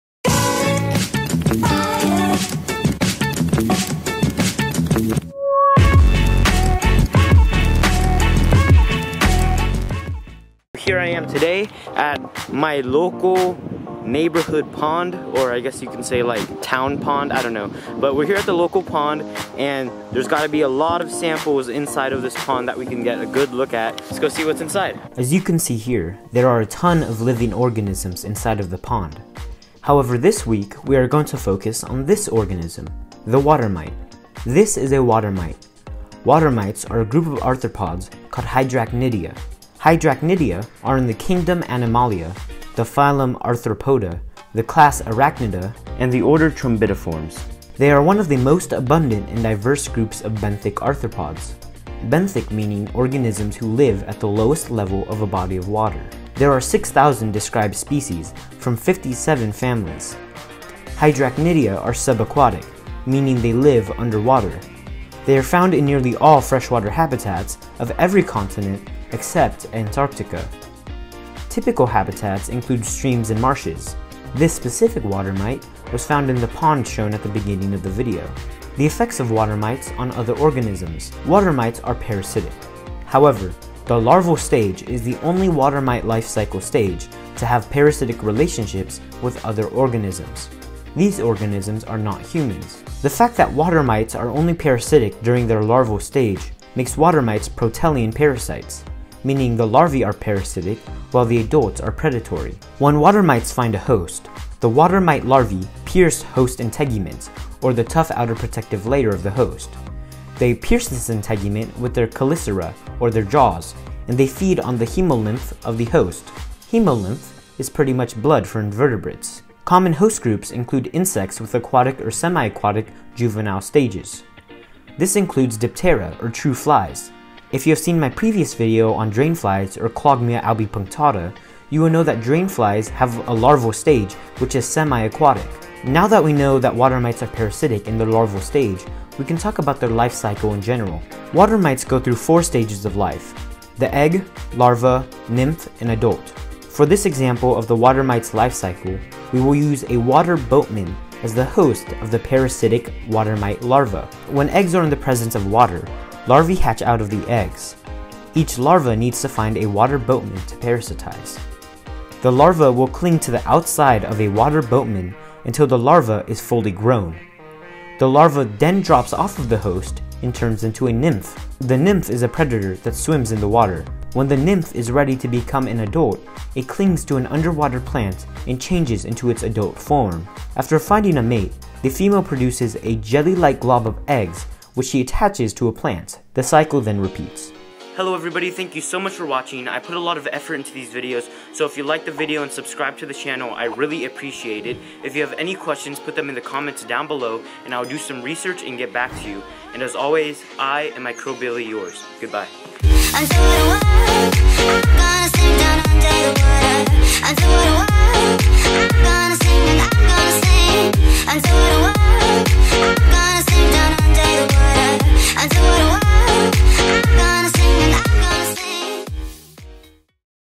WATER MITES UNDER A MICROSCOPE (Hydrachnidia) | Microscope Monday
In this episode of Microscope Monday we take a look at water mites Under a Microscope. We also learn about what water mites are!
I used the Olympus cx21 microscope in this video:
I filmed with my phone on a tripod.
Additional Information (Sources at bottom):
This is a water mite. Water mites are a group of arthropods called Hydrachnidia.
Hydrachnidia are in the kingdom animalia, the phylum Arthropoda, the class Arachnida, the order Trombidiformes or acarina, and are the family Hydrachnidia. They are also in the group Parasitengona which is a taxonomical rank found between order and family.

They are one of the most abundant and diverse groups of benthic arthropods (benthic meaning organisms who live at the lowest level of a body of water). There are 6,000 described species from 57 families.[2] Hydracarina are subaquatic, meaning they live underwater. They are found in nearly all freshwater habitats. of every continent except Antarctica. Typical habitats include streams and marshes, this water mite was found in the pond shown at the beginning of the video.

Effects on Humans:
Water mites are parasitic, however, the larval stage is the only water mite life cycle stage to have parasitic relationships with other organisms. NOT HUMANS. This makes water mites protelean parasites meaning the larvae are parasitic while the nymphs and adults are predatory. Upon location of a host, larvae pierce host integument (or their tough outer protective layer) with their chelicerae (jaws) and feed on hemolymph (blood for invertebrates) until fully engorged or brushed off.  Common host groups include insects with aquatic or semi-aquatic juvenile stages, including, but not limited to, the Diptera (true flies), Odonata (dragonflies and damselflies), and Trichoptera (caddisflies). In some cases, high infection intensities have significantly increased chances of host mortality and reduced fecundity. Water mite larvae have been considered as a potential biological control agents, although low natural infection intensities warrant supplementation with other control strategies in order to be effective.
Life Cycle:
Life cycle: Water Mites go through four stages of life: egg, larva, nymph and adult. When a vernal pool fills with water, the larvae hatch out of the eggs. Each larva needs to find a Water Boatman to parasitize. The larva will cling to the outside of a Water Boatman until the larva is fully grown. Then it drops off the host and turns into a nymph. The nymph is a predator that swims in the vernal pool. When the nymph is ready to become an adult, it clings to an underwater plant and changes into its adult form. After finding a mate, the female produces a jelly-like glob of eggs, which she attaches to a plant. Then she dies. The eggs remain there as the pool dries up. They are adapted to survive the long, hot summer. When the winter rains fill the vernal pool, the Water Mite repeats its life cycle.
Sources:
My videos are educational relating to biology, microscopy, and medicine.